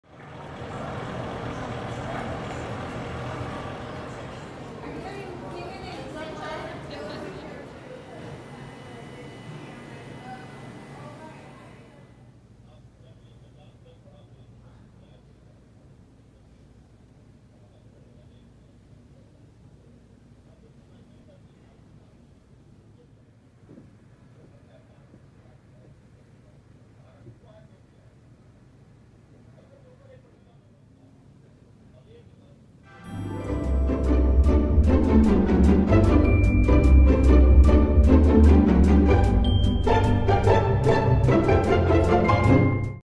Shopping at PIKE PLACE MARKET • Seattle, Washington • Kitchen Basics • Washington Apples
Here's a quick video from Seattle's Pike Place Market. I needed a new kitchen towel. And, in honor of Washington Apples... I found one!

I know @IntheKitchenwithKaren & @RhubarbAndCod would really dig this kitchen store!